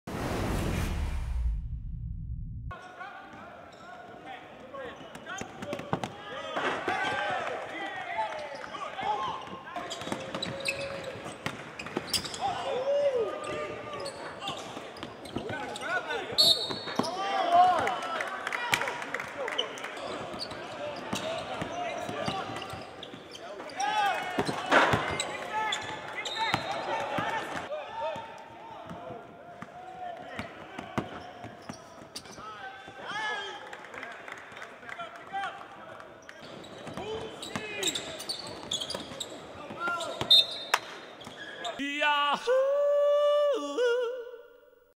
NBPA Top 100 Camp: INTERNATIONAL Small Forward Tadas Sedekerskis Highlights
Highlights from the 2016 NBPA Top 100 Camp in Virginia. Tadas Sedekerskis plays for Penas Huescas (Spain) and is from Lithuania.